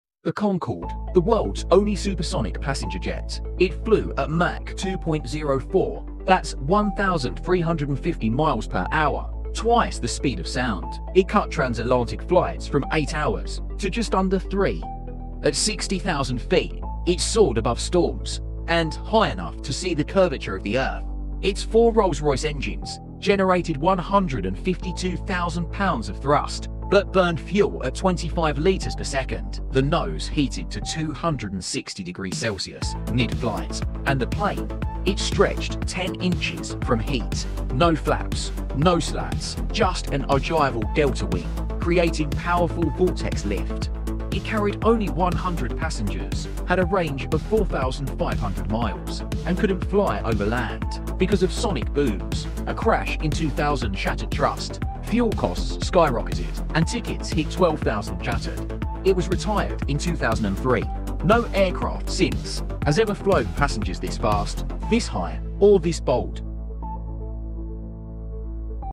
Why the Concorde Was Too Perfect to Survive?
Meet the Concorde: an Anglo-French Commercial passenger Flight miracle that could fly from New York to London in under 3 hours. Its ogival delta wing, droop nose, and Rolls-Royce engines made it a masterpiece. But fuel thirst, a crash, and sonic boom laws ended its reign. This is the rise and fall of the fastest jet to ever carry passengers.